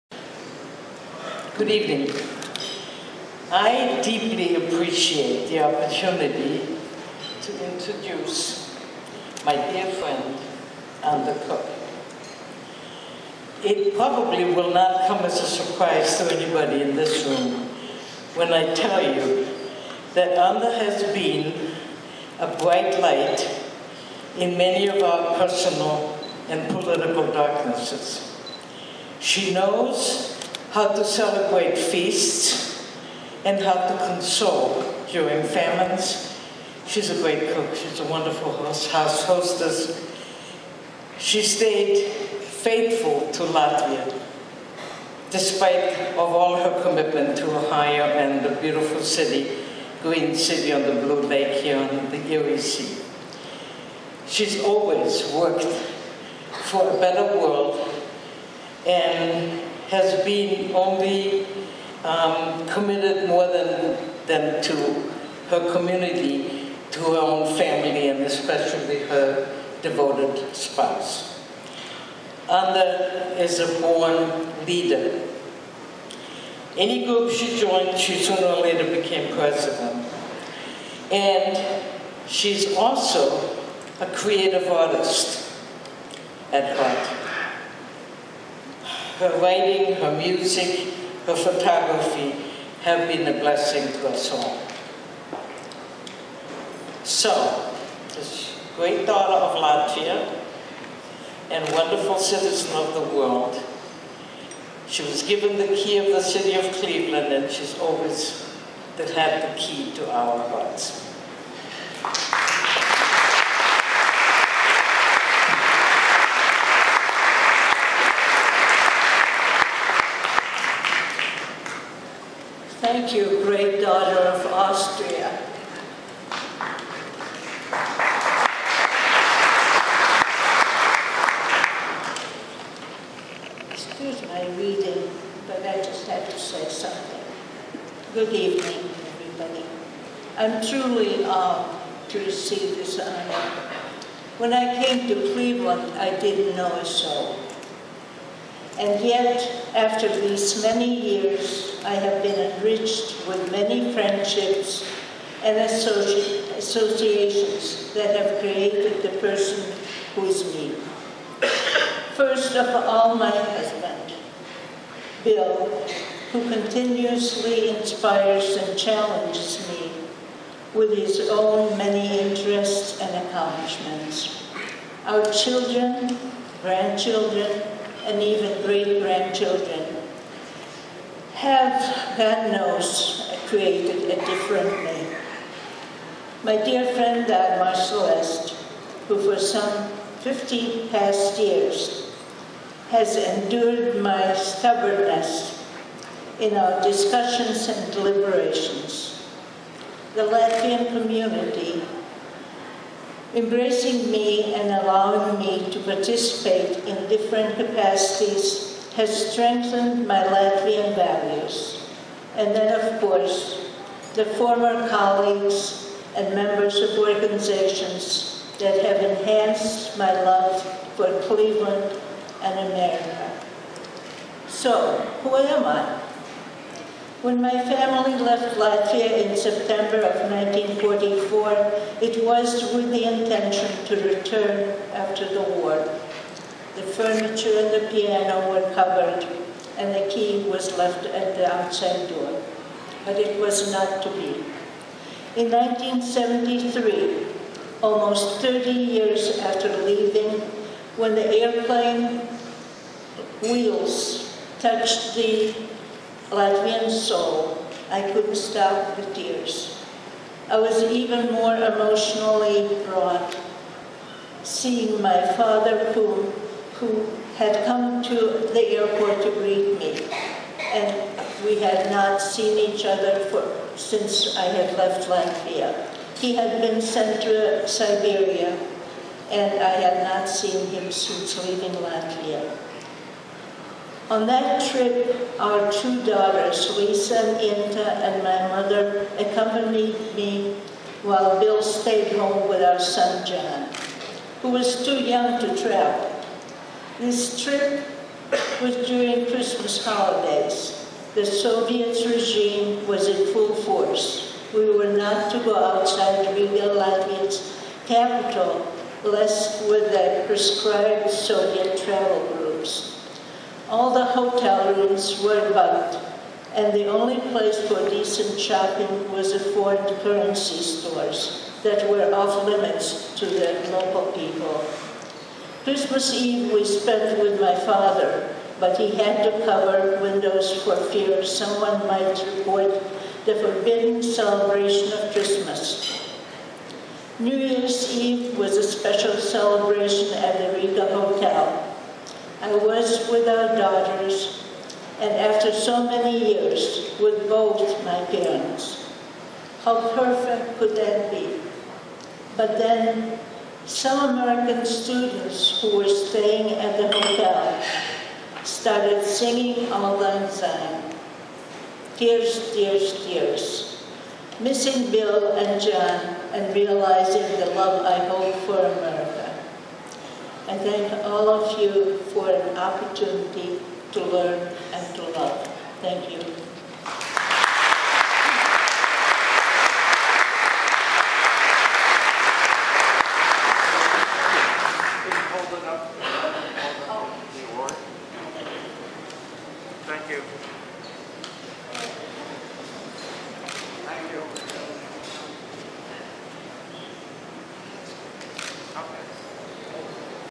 Anda Cook inducted by Dagmar Celeste into Cleveland International Hall of Fame
The induction ceremony for the Cleveland International Hall of Fame (CIHF) took place on Tuesday June 7 at Windows on the River in Cleveland. Dagmar Celeste, executive director of the Tyrian network, inducted Anda Cook. Anda Cook was born in Riga, capitol of Latvia, but when she was 9 years old, left with family to escape the advancing Soviet army.  She has a long and impressive list of accomplishments and awards including working to establish the Latvian Cultural Garden and serving as vice president on the Board of the Cleveland Cultural Gardens Federation. Anda led the iconic One World Day in the Cleveland Cultural Gardens for several years.